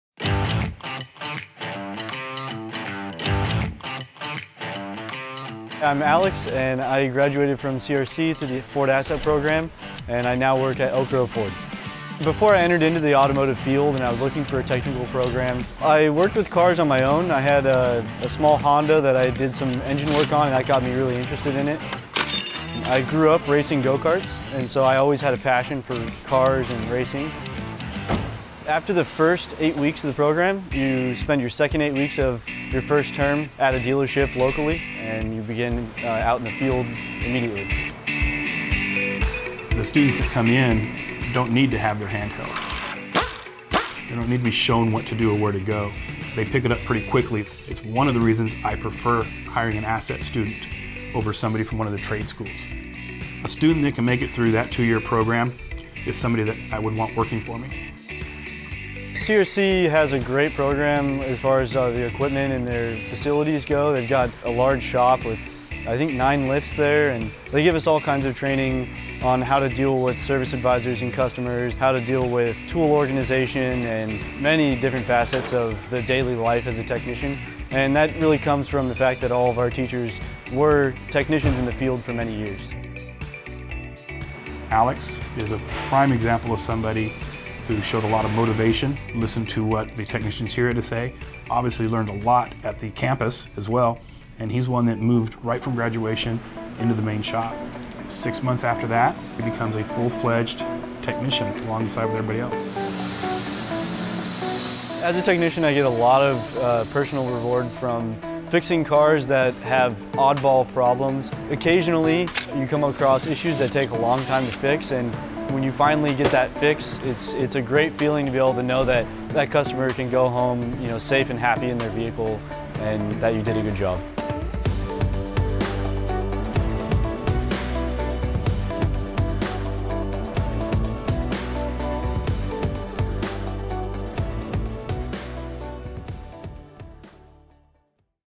Ford ASSET Alumni
Ford ASSET stands for the Automotive Student Service Educational Training Program. It is a partnership between Ford Motor Company, Ford, Lincoln dealers, and local participating community colleges.